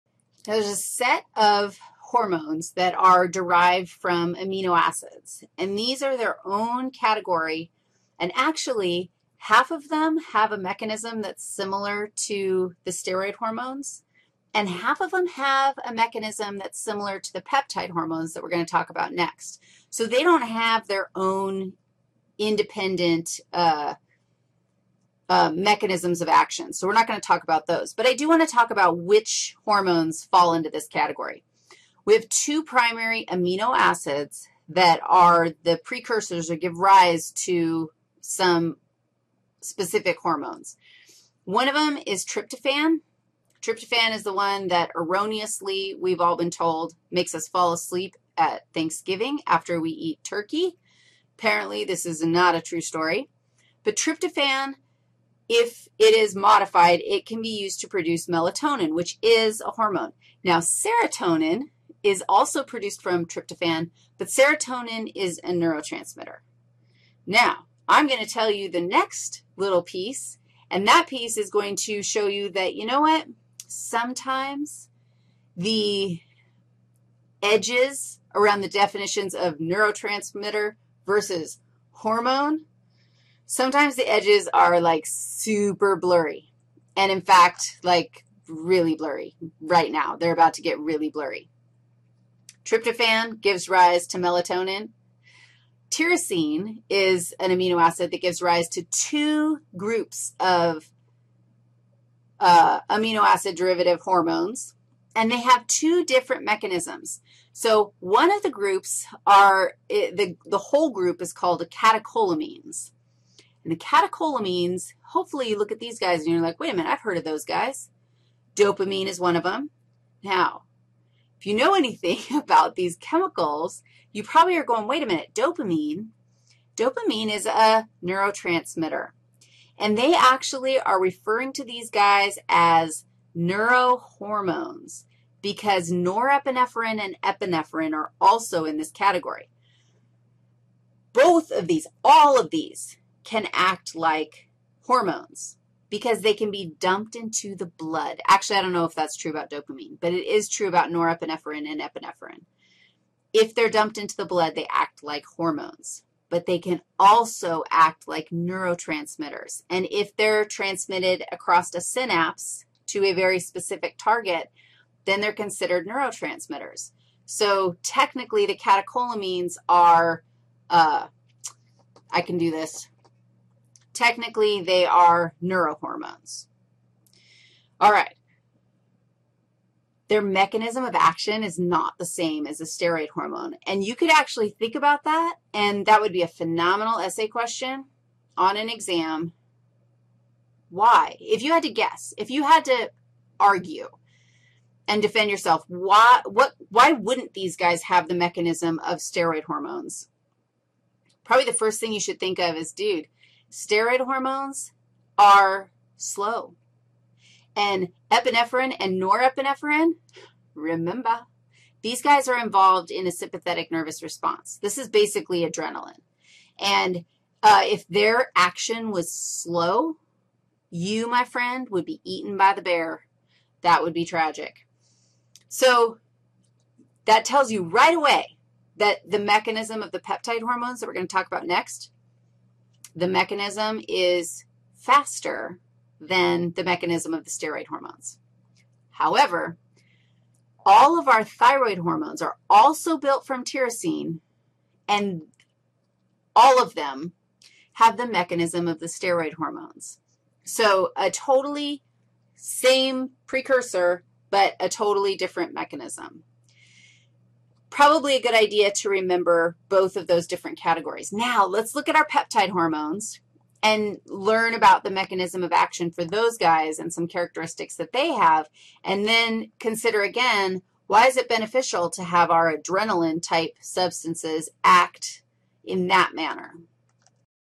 Endocrine 5- Amino acid derivatives
Part 5 in a 8 part lecture on ENDOCRINE SYSTEM in a flipped Human Physiology course taught by Wendy Riggs. CC-BY.  Watch the whole lecture (all 8 videos) by going to the PLAYLIST
Some images from Human Physiology: An Integrated Approach (4th Edition) by Dee Silverthorn.